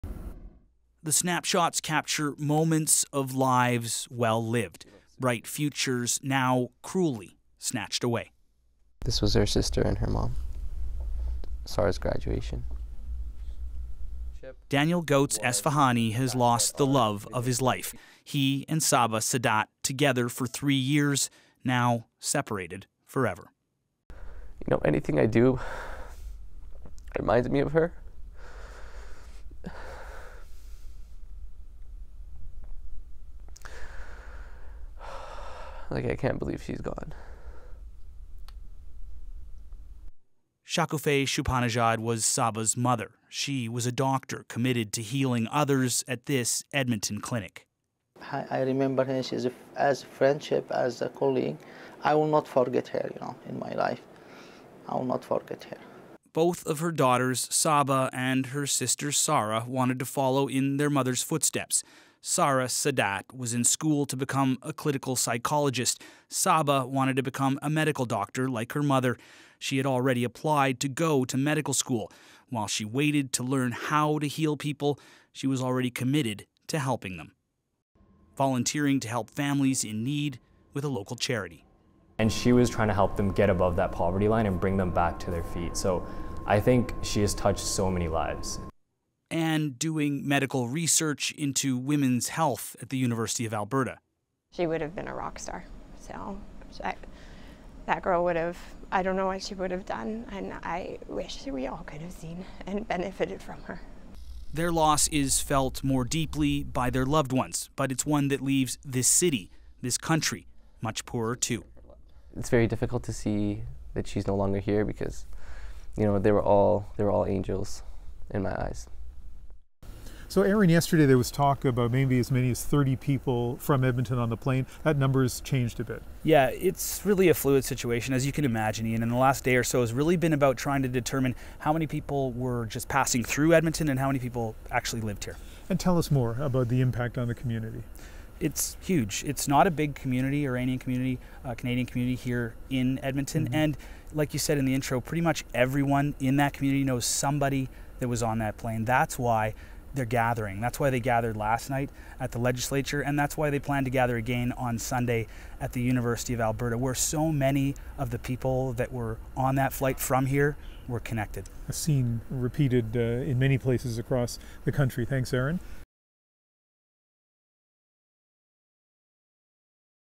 Mother, two daughters killed on Flight 752
A mother and her two daughters from Edmonton killed in the crash are remembered for their shared passion to help people.